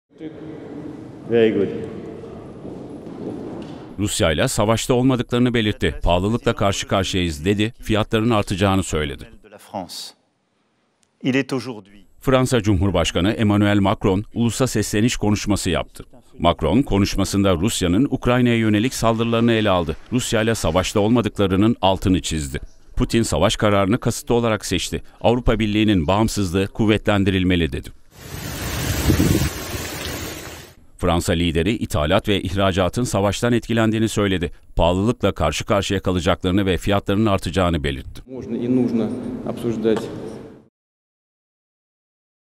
Macron ulusa sesleniş konuşması yaptı! Rusya ile ilgili açıklamalarda bulundu
TÜRKSAT 4A
Frekans: 12729
Sembol: 30000
Polarizasyon: V (Dikey)